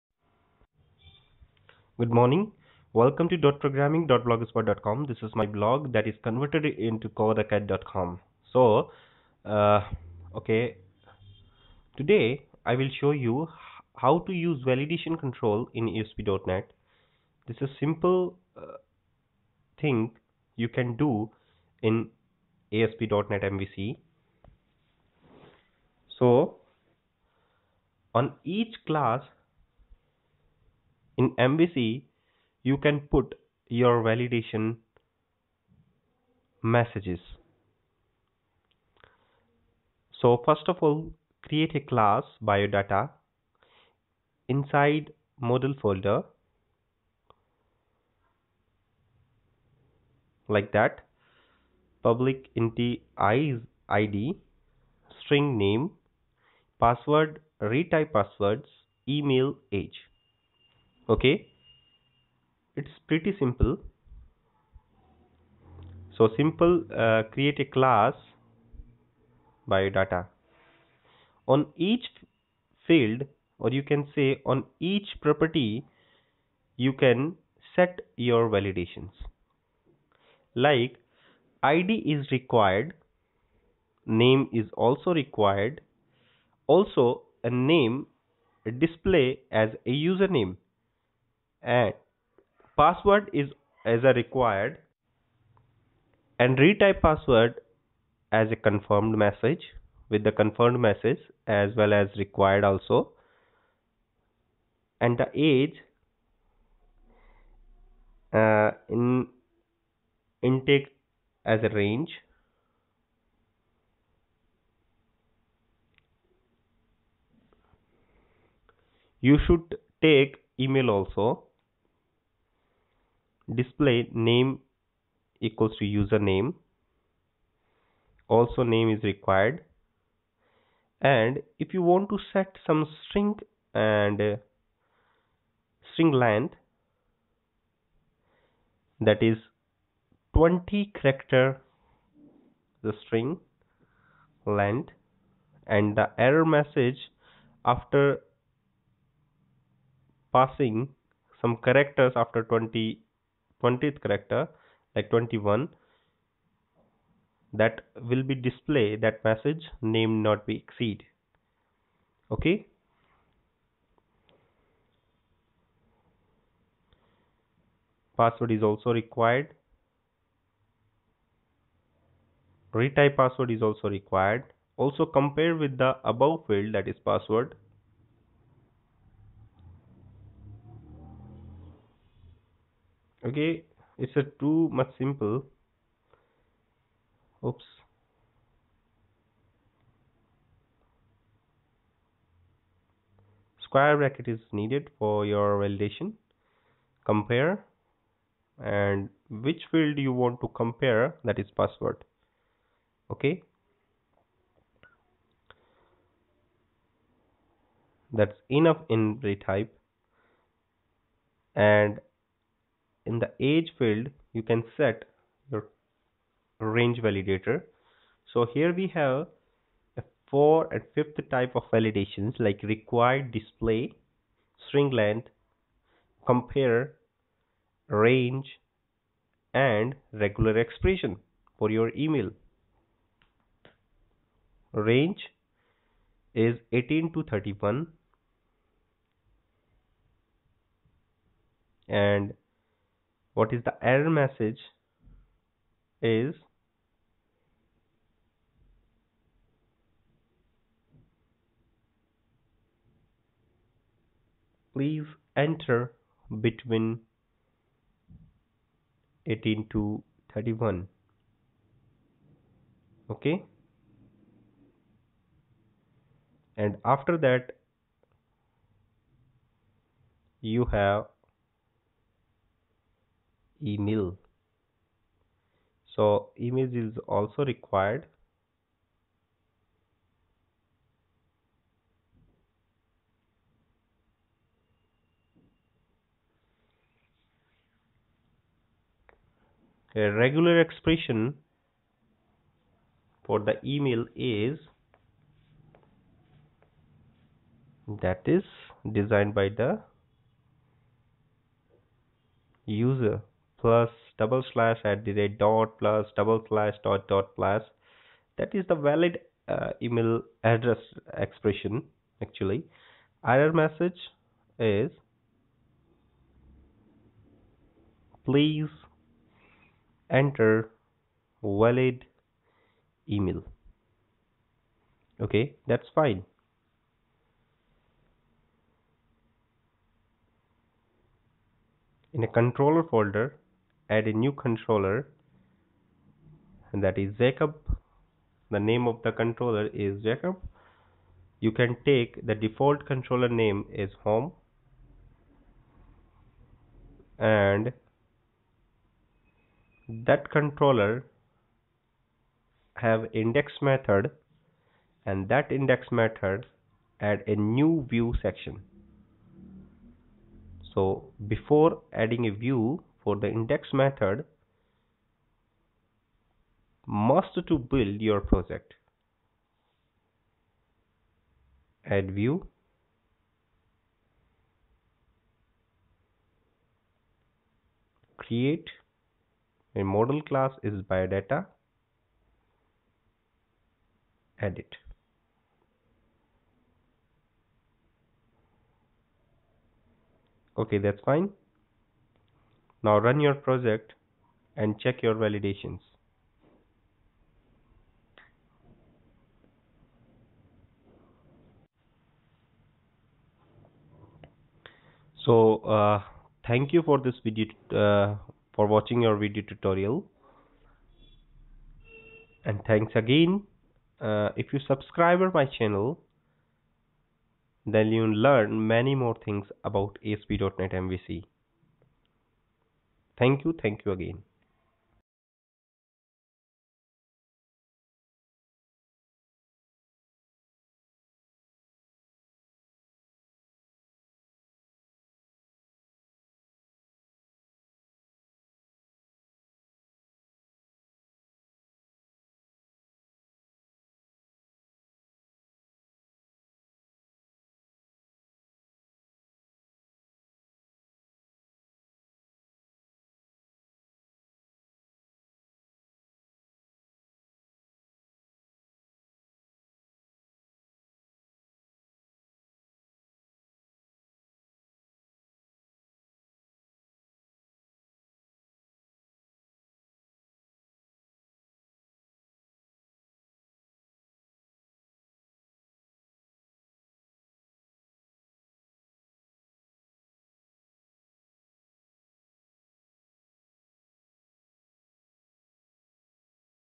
Validation in ASP.NET MVC | Client Side Validation
In this ASP.NET MVC Video tutorial i will show you , How to use validation in ASP.NET MVC.  I come from ASP.NET WEB FORM , So, i share same validation in mvc , thease are:
1. Required Field Validator
2. Rangle Field Validator
3. Compare Field Validator
4. Regular Field Validator
5. StringLength validator.